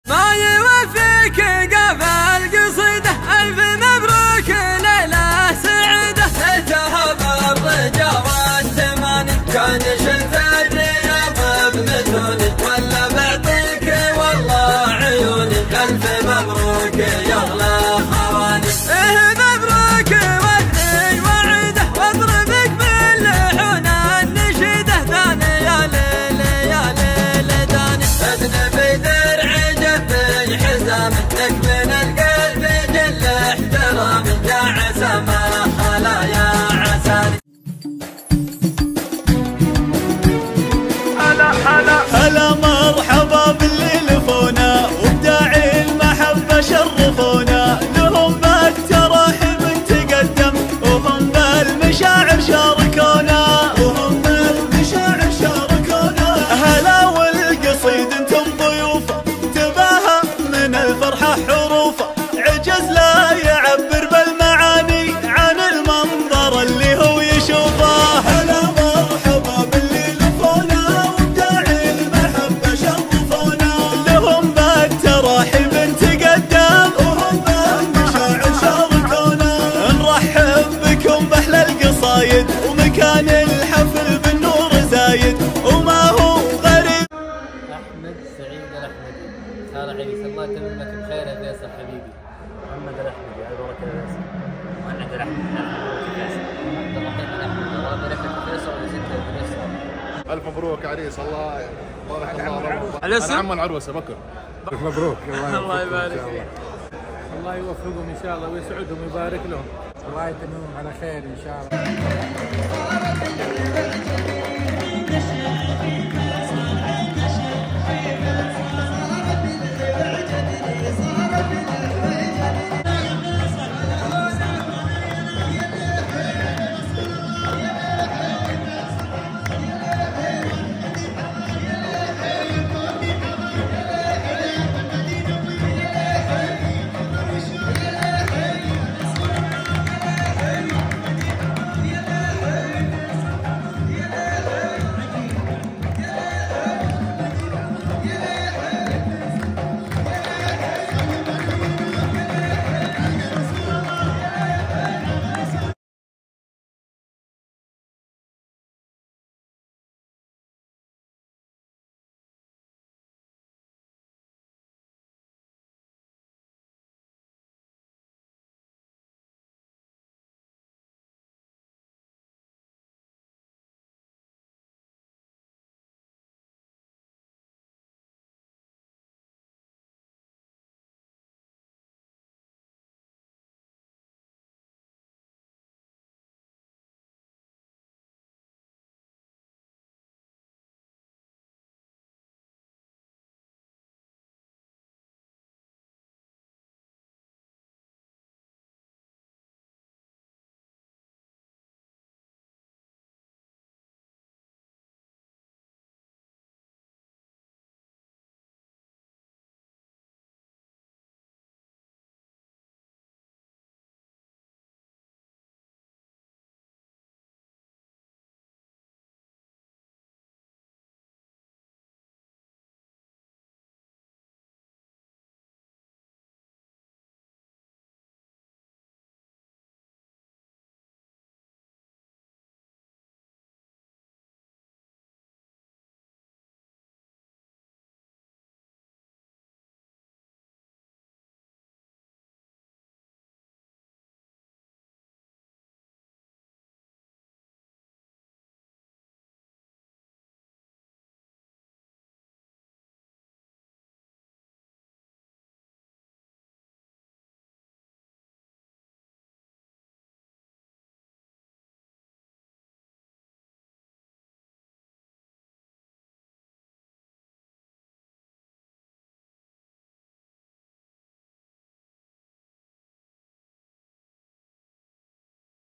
بروموا حفل الكاتب فهد عامر الاحمدي بمناسبة زواج ابنه فيصل
مصدر الشيلة  الاولى شيلة ترحيبية زواجية — اداء المنشد : حسين ال لبيد

حفل الكاتب فهد عامر الاحمدي
بمناسبة زواج ابنه فيصل

بروموا و صور على التغرام